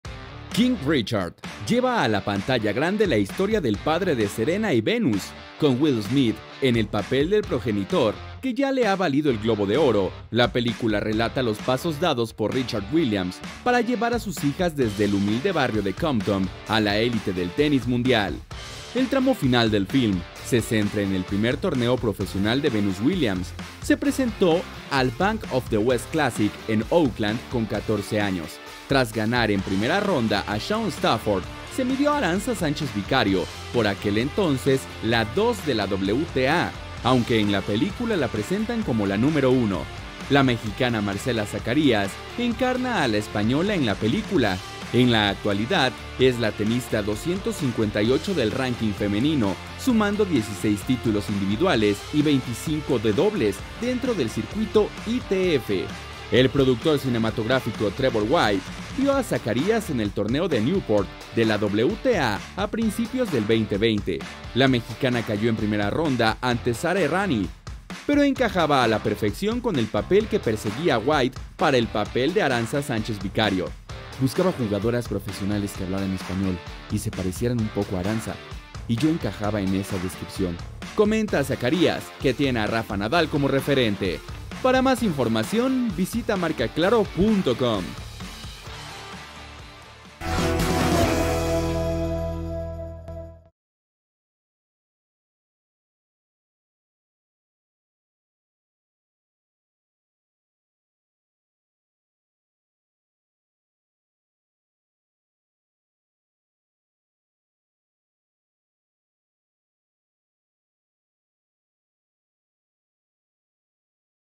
La tenista mexicana Marcela Zacarías, de la pista al cine en 'King Richard'
King Richard, lleva a la pantalla grande la historia del padre de Serena y Venus. Con Will Smith en el papel del progenitor, que ya le ha valido el Globo de Oro, la película relata los pasos dados por Richard Williams para llevar a sus hijas desde el humilde barrio de Compton a la élite del tenis mundial.